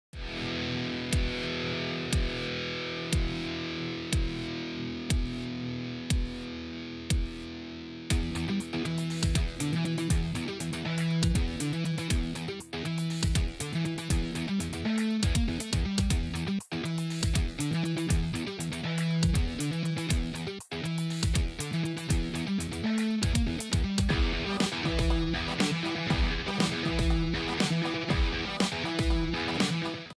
Ski Town of Breckenridge, Colorado at Twilight - Aerial Drone View
Aerial drone video of the ski town of Breckenridge, Colorado at twilight. DJI Mavic 2 Pro.